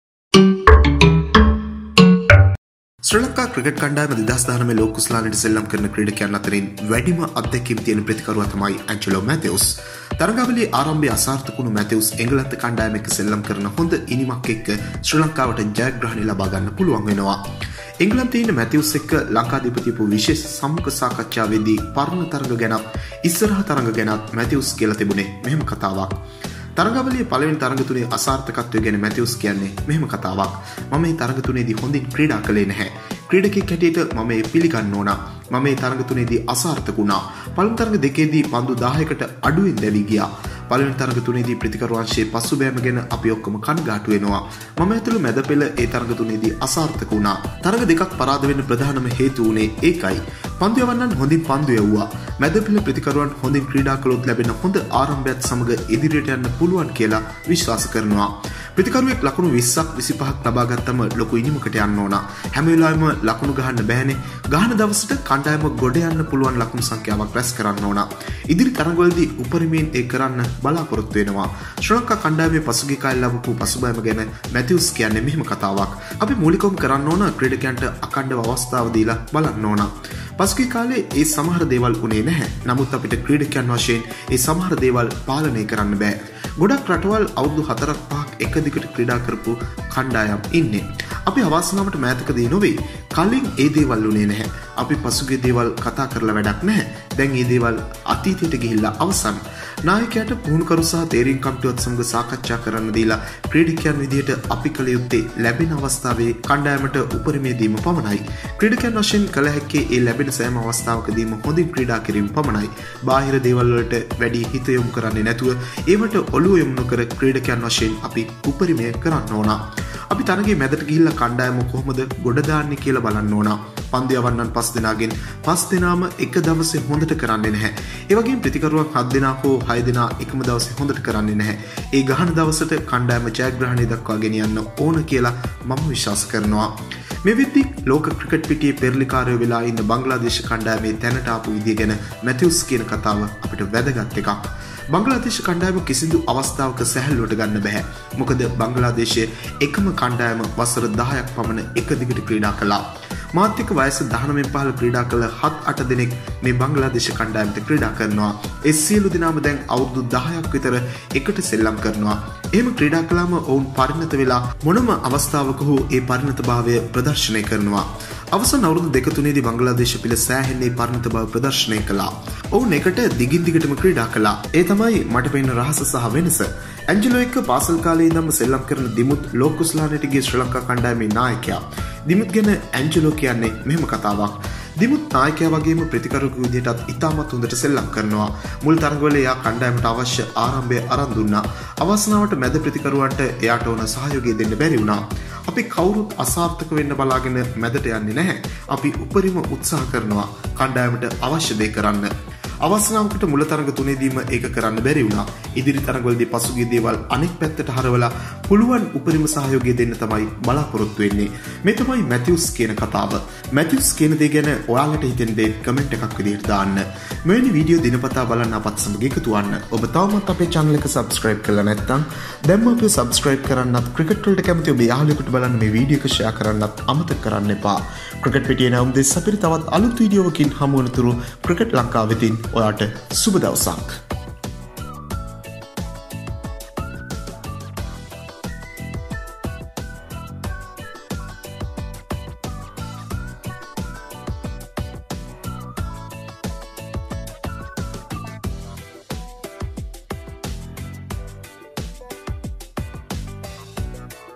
දිමුත්ගේ වැඩ ගැනත්, ඉස්සරහට කරන්න ඕන දේ ගැනත් මැතිව්ස් කියන දේ මෙන්න..
2019 ලෝක කුසලානයට ශ්‍රී ලංකා කණ්ඩායම යන්නේ තරඟාවලියට යන දුර්වලම කණ්ඩායමක් විදියට,හැබැයි තරගාවලියේ ටික දුරක් යද්දී අපි අවසන් කණ්ඩායම් 4 අතරට යන සටන ගැන හිතන්න පුළුවන් මට්මට එනවා..